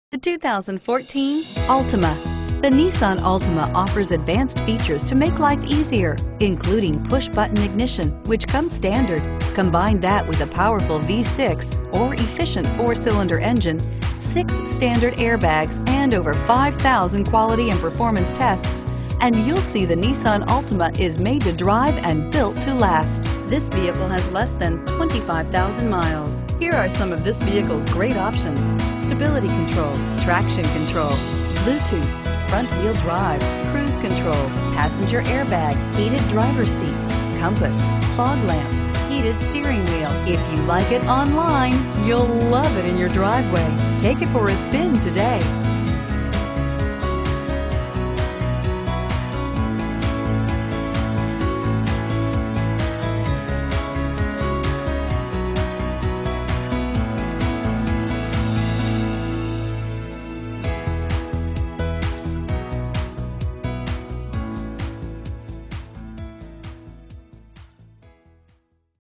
2014 Nissan Altima Chico CA U7565C
2014 Nissan Altima 2.5 SL

Looks Fantastic! Certified! Bluetooth, Heated Front Seats, Leather Seats, and CVT Auto Trans. 38.0 MPG! This near new Nissan Altima SL has a great looking Blue exterior and a Charcoal interior!  Our pricing is very competitive and our vehicles sell quickly. Please call us to confirm availability and to setup a time to drive this Altima!